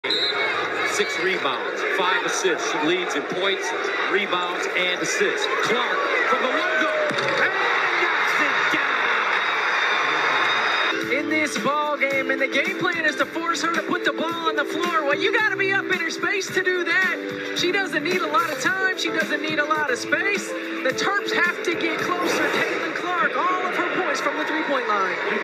Caitlin Clark Hits Big LOGO Three
Caitlin Clark Hits Big LOGO Three Pointer NCAAW Iowa vs Maryland February 3, 2024